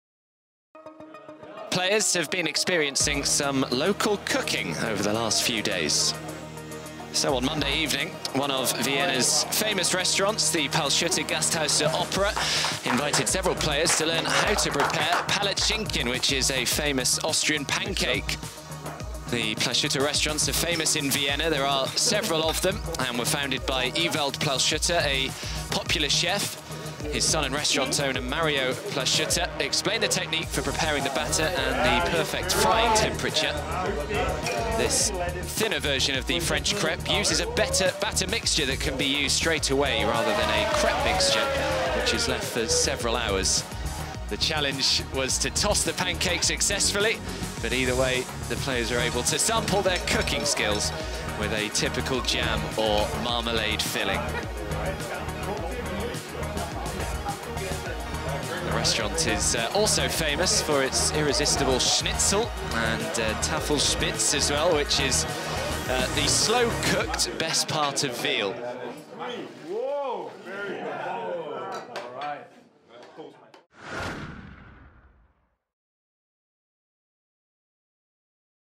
Dimitrov, Tiafoe Test Pancake Flipping Skills In Vienna 2018
Watch as Grigor Dimitrov, NextGenATP American Frances Tiafoe and a number of ATP World Tour stars enjoy a pancake-making lesson during the Erste Bank Open 500 in Vienna. Watch live tennis at tennistv.com.